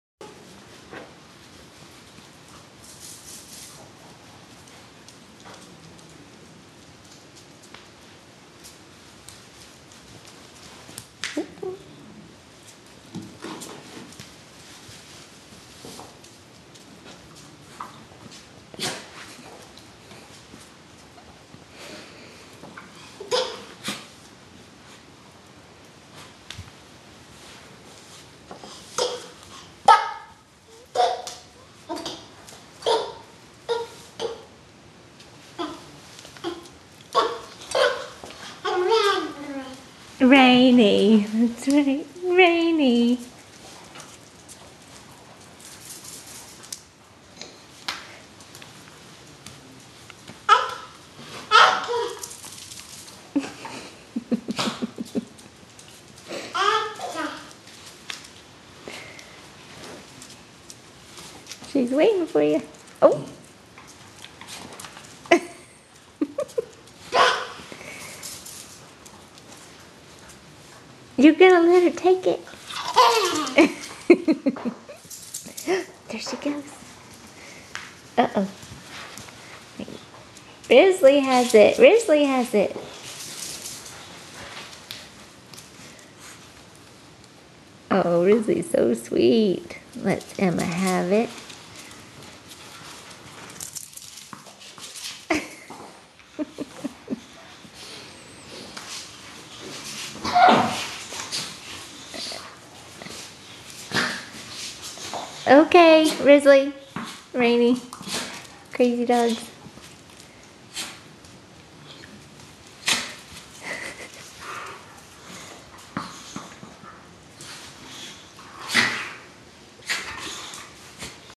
toddler playing ball w dogs
I don't know if it's that Emma thinks she's one of the dogs, or the dogs think they are one of the people. Either way, they are BFFs!